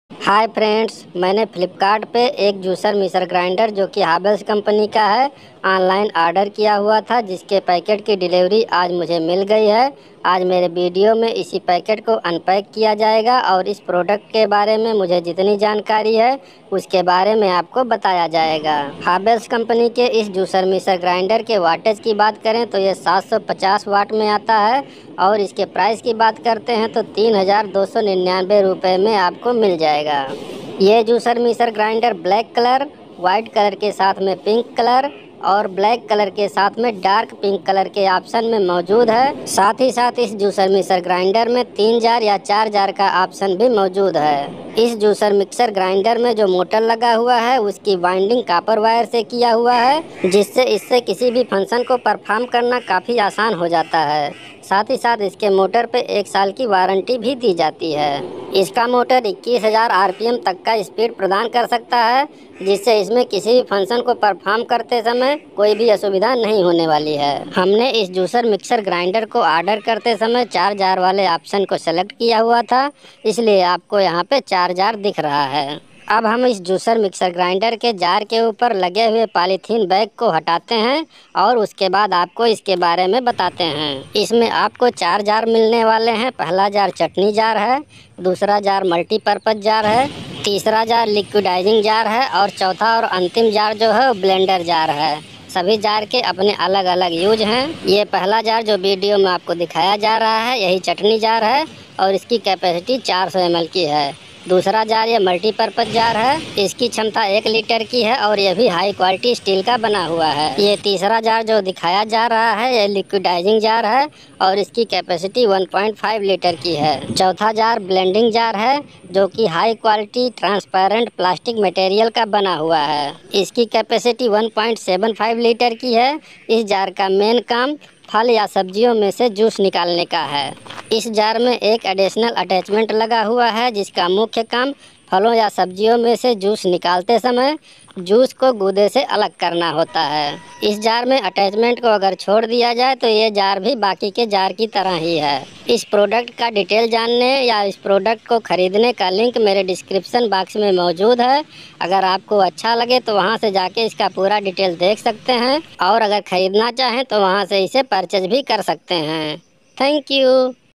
Havells Juicer Mixer Grinder l Klassic 750W 4 Jar l Juicer Mixer Grinder unboxing and review
Hi friends, you are welcome in my channel.

My video is about to unboxing and review of "Havells Klassic 750W Juicer Mixer Grinder (4 Jars, White - Lavender)" purchased online at Flipkart shopping with price ₹3299/- only.

Product related link